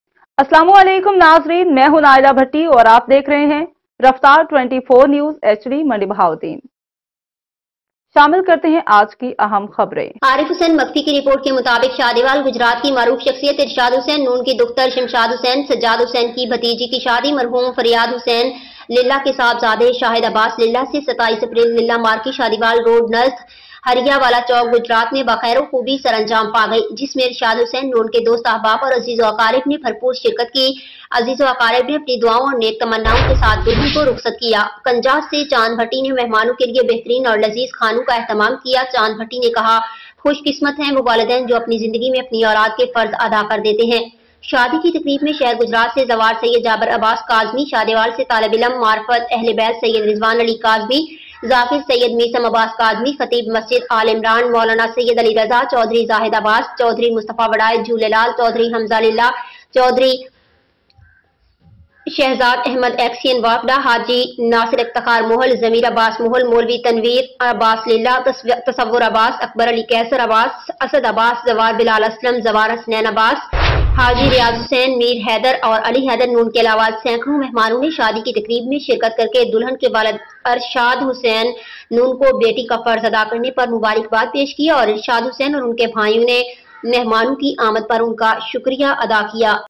27 اپریل للہ مارکی شادیوال روڈ گجرات ارشاد حسین نون آف شادیوال کی بیٹی کی شادی رفتار 24 نیوز رپورٹ۔
Video from arifhussainmakhfi